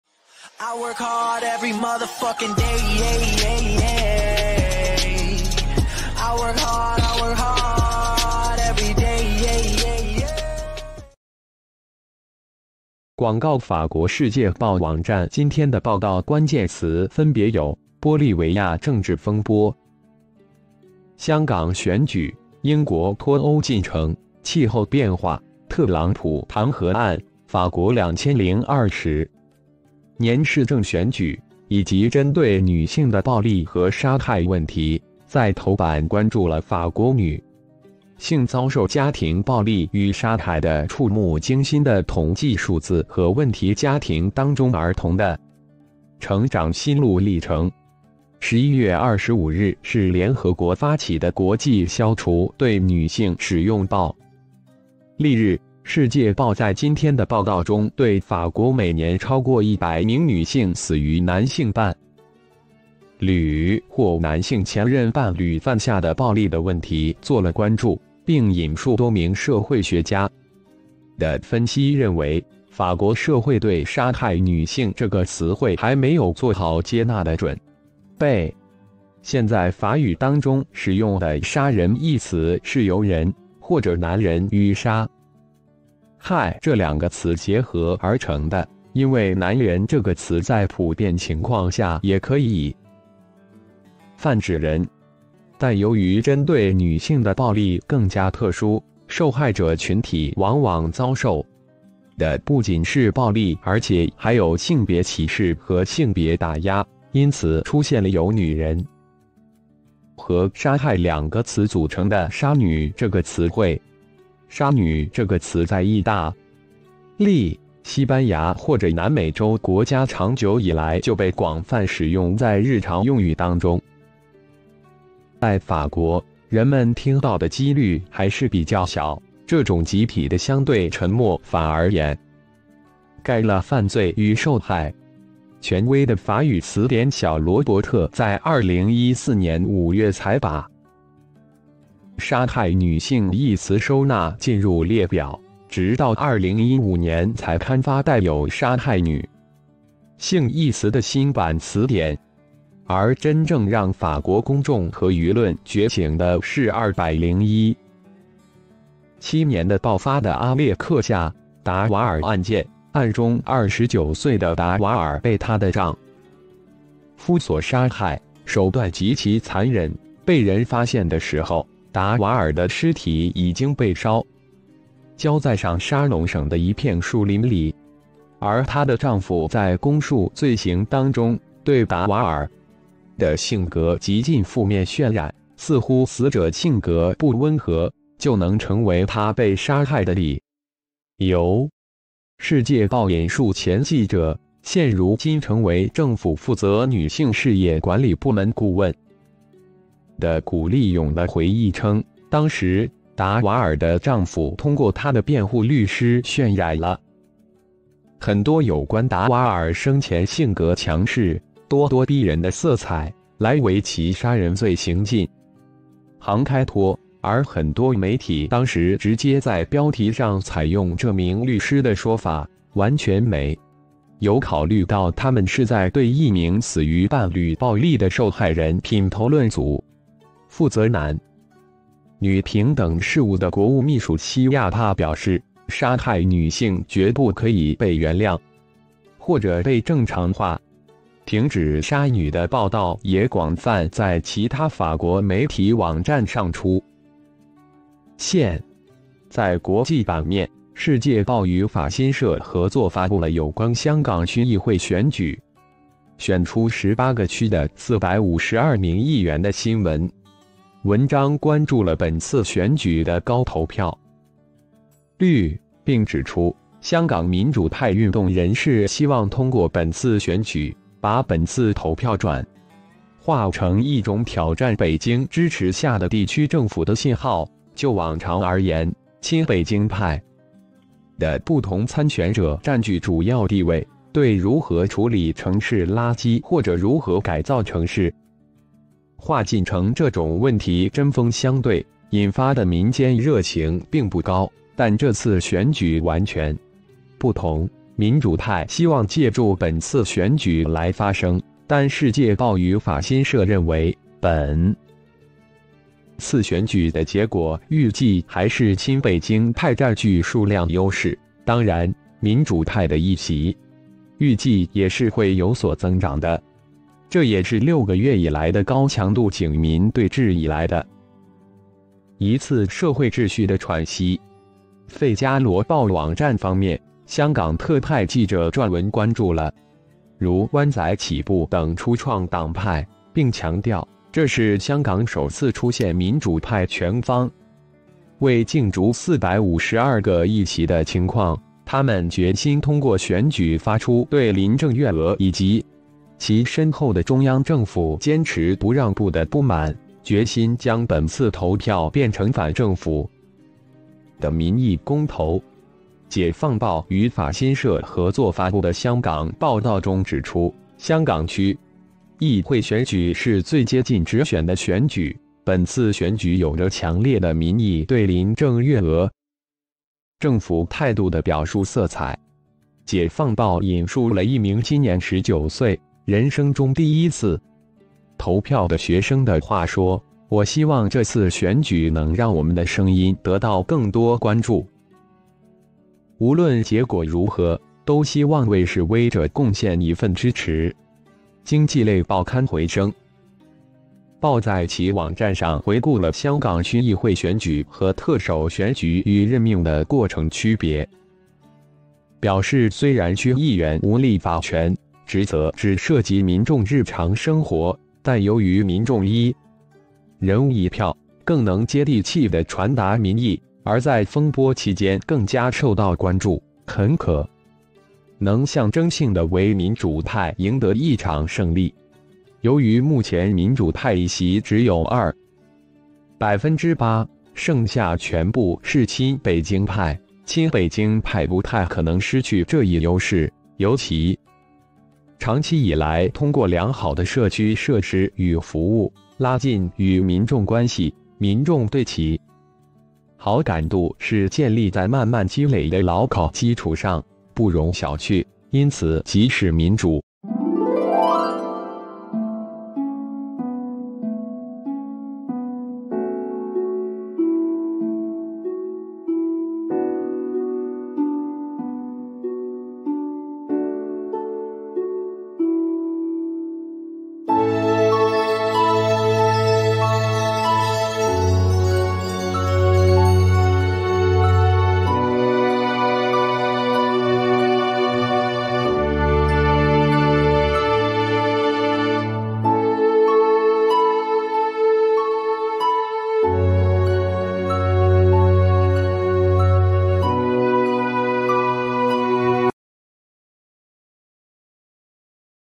香港区议会选举:民主派追求的首先是象征性的
今天是2019年11月24日星期天，今日白天我们没有收到订阅的纸质刊物，因此在本台订阅的网站上选取今天的重点内容进行摘要。